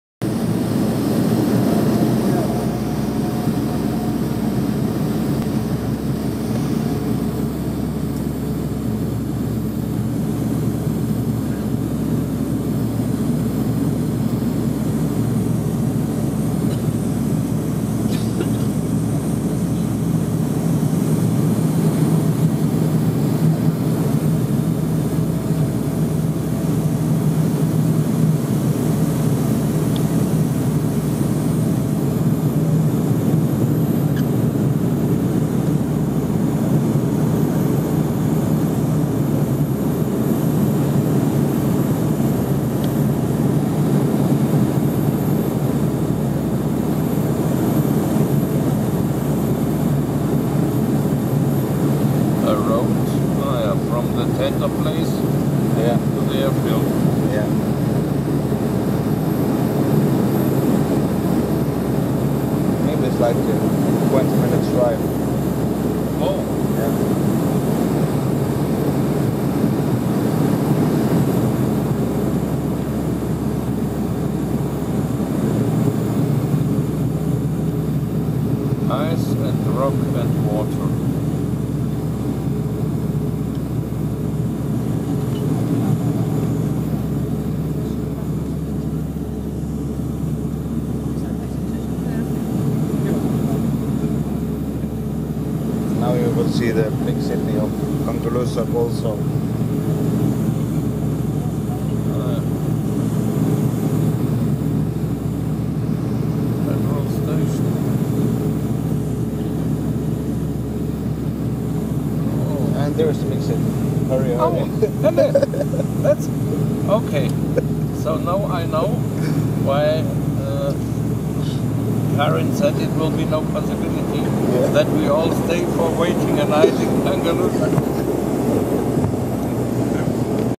Kangerlussuaq, Søndre Strømfjord (SFJ), Final descent and landing, Runway 10
Nuuk (GOH) - Kangerlussuaq (SFJ), 2015|06|13, Final descent and landing in Kangalussuaq, GL 6627, Dash 8, Runway 10